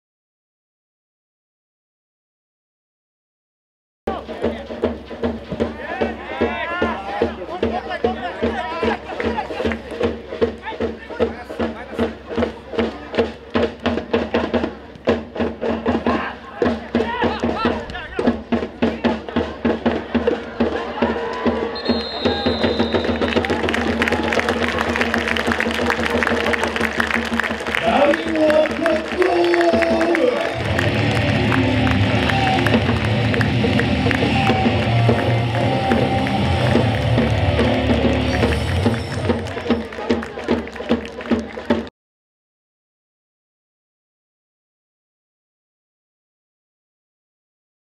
【GOALハイライト】'89 奥直仁（第48回北信越フットボールリーグ1部 第1節）
2022/4/10（日）
第48回北信越フットボールリーグ1部 第1節
福井ユナイテッドFC vs. リベルタス千曲FC　7-0

試合結果はこちら！